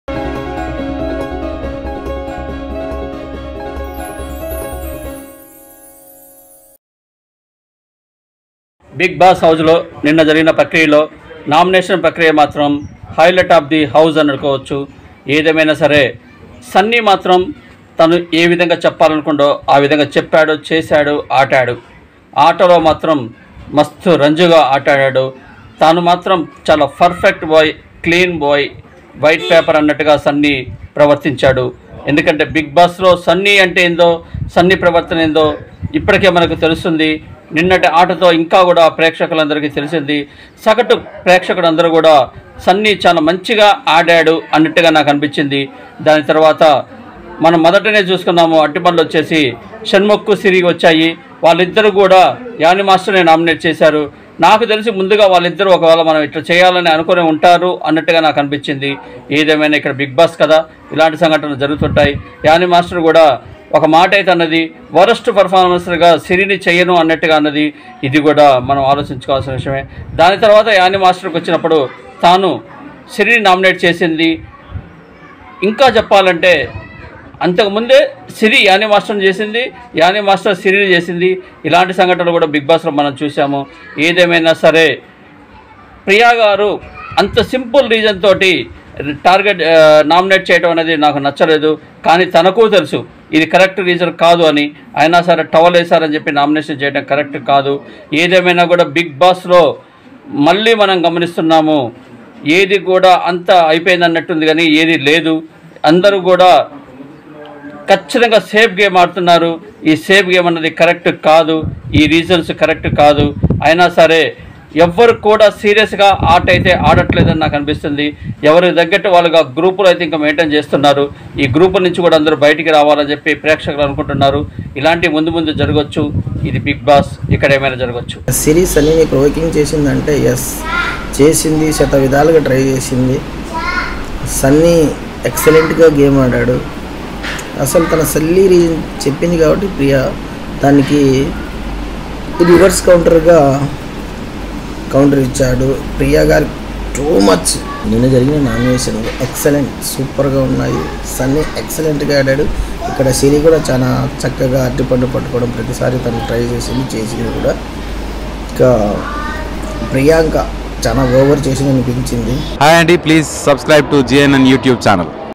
Public Talk on Sunny Decision l Bigg Boss Telugu Season 5 Public Talk l GNN TV Telugu
public response on 6th week nominations.